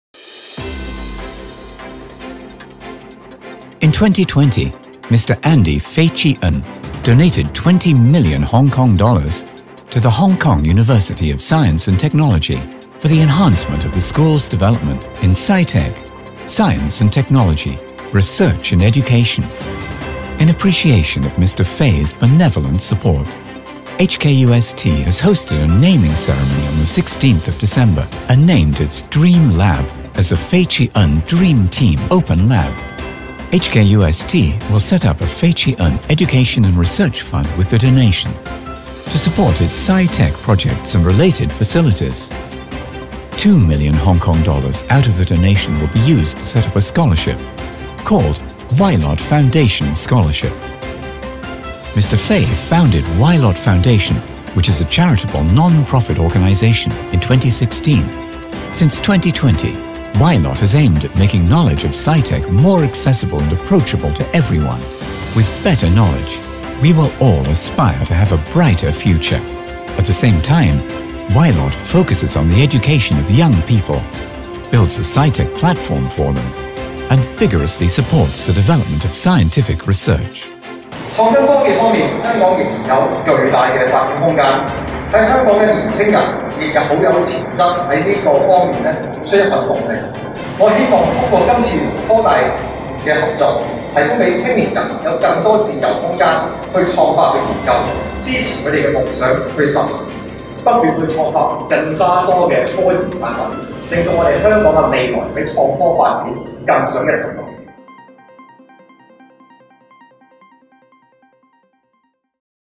Y-LOT主席費志恩向科大捐贈2000萬 支持年青人科創研究及發展👍
【科大成立「費志恩教育及科研基金」】

Y-LOT創辦人兼主席費志恩先生個人捐款2000萬元港幣，支持科大科創研究及年青人創科發展，其中200萬設立「晴朗的心獎學金」獎勵成績優異大學生，科大亦將校內的「夢想實驗室」命名為「費志恩夢想啟航基地」。👍🥳
𝘔𝘢𝘬𝘦 𝘪𝘵 𝘩𝘢𝘱𝘱𝘦𝘯, 𝘠-𝘓𝘖𝘛!